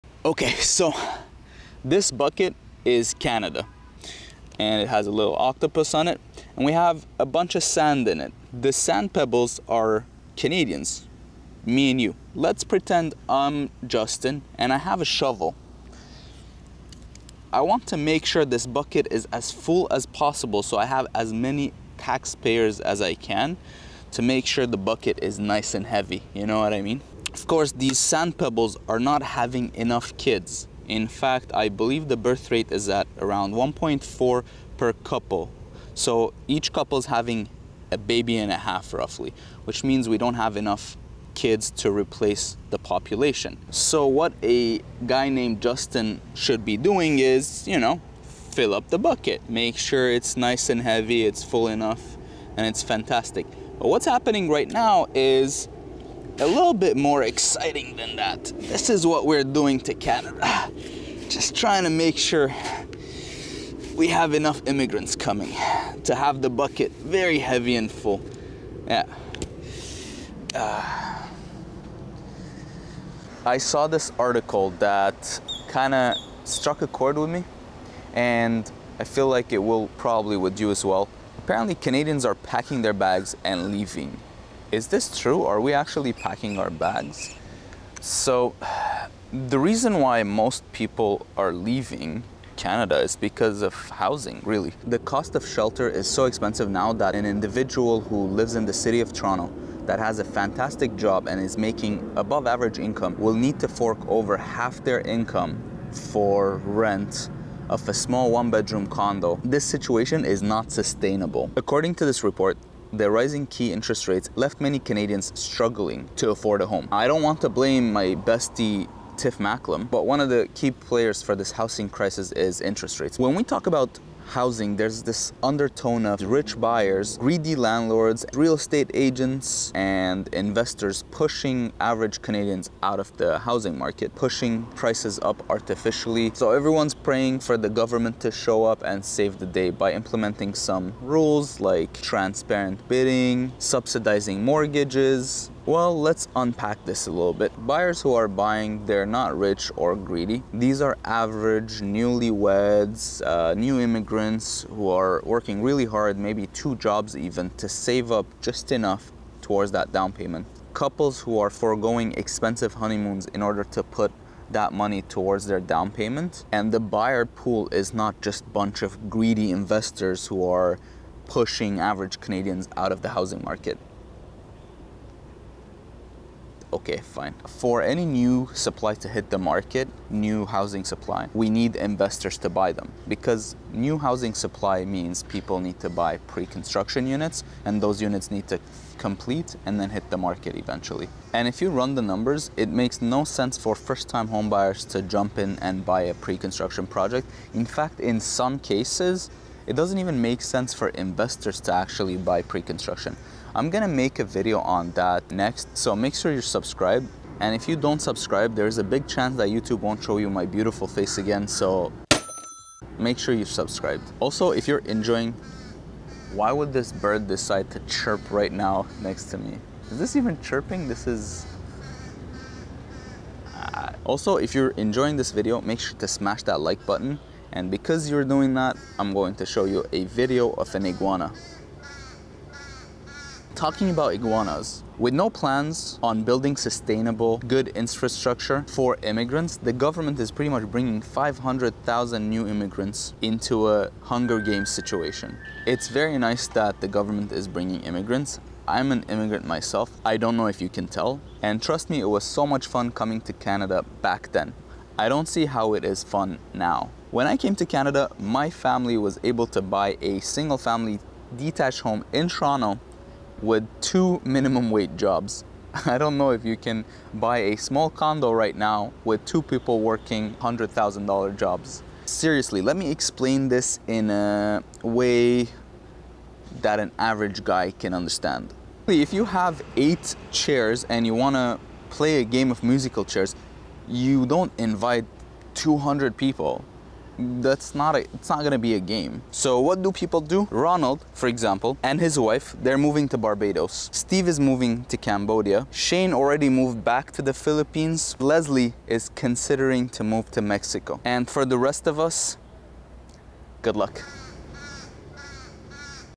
Why everyone leaving Canada?
Housing is so expensive Canadians are choosing to leave the country. Yet, the government is loading it up with immigrants with no regards to the economic ability of the country to handle it. Maybe government will succeed since it's way harder to pack up and leave. Won't match the rate of 500,000 people coming each year. I'm sure we'll get used to it.

Good luck to us all.

'I'm done with Canada': High cost of living leads some to leave the country - CTV

Investors now make up more than 25% of Ontario homebuyers, pushing prices higher, experts warn - CBC